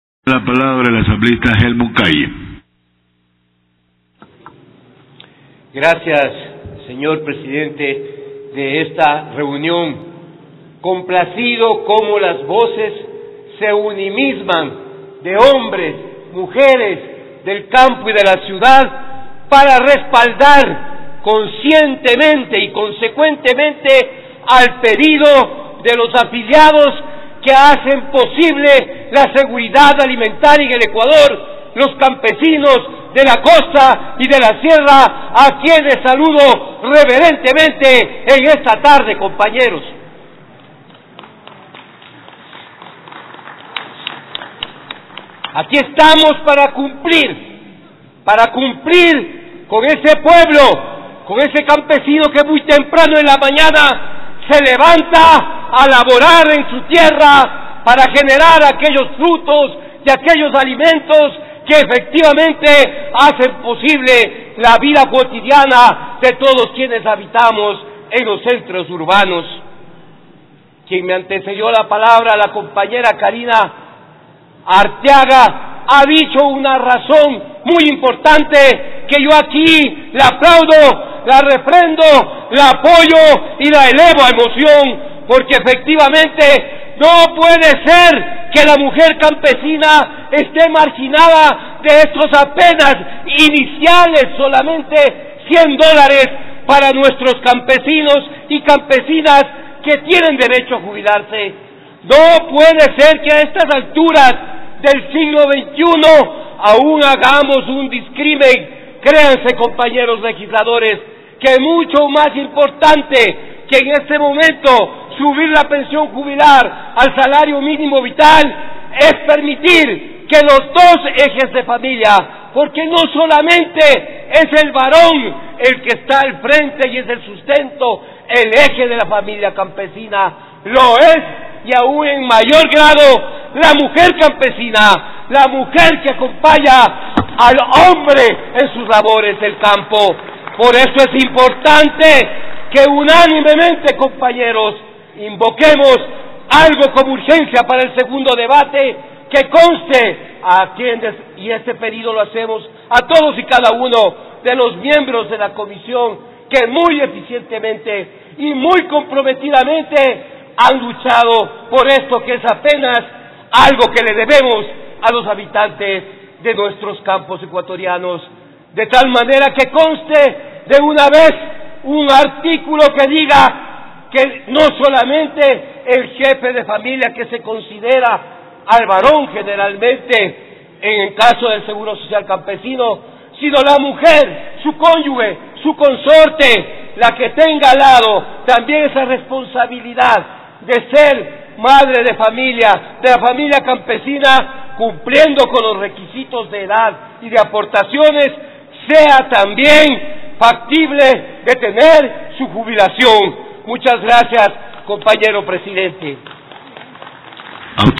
Hermuy Calle - Sesión 497
15 de febrero de 2018
Primer Debate del Proyecto de Ley Reformatoria a la Ley de Seguridad Social.